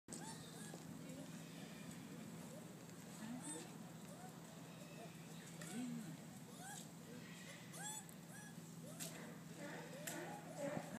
東山動植物園：ヤブイヌの鳴き声
東山動植物園の"ヤブイヌ"を撮影した動画。撮影には iPhone 4S を使用。ヤブイヌは動物園の説明書きによると「もっとも原始的なイヌ科動物の一種」との事ですが、その鳴き声はまったく"犬"らしくありませんでした。(^^;